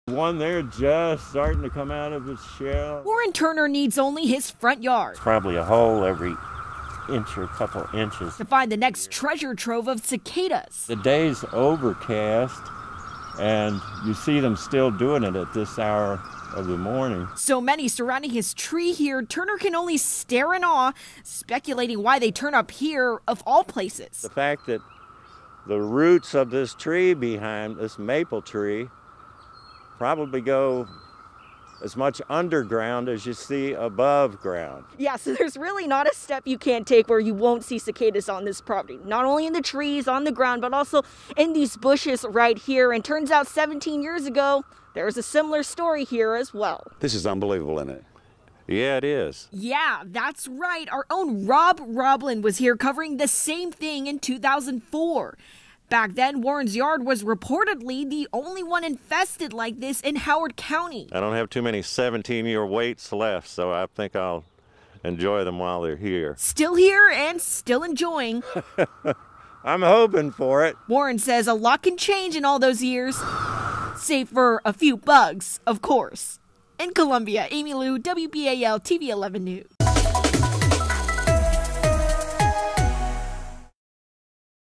Columbia tree infested with cicadas in 2004 struck again in 2021
Much can happen in 17 years, but one Columbia man is happy to report few things have changed with the cicadas in his front yard.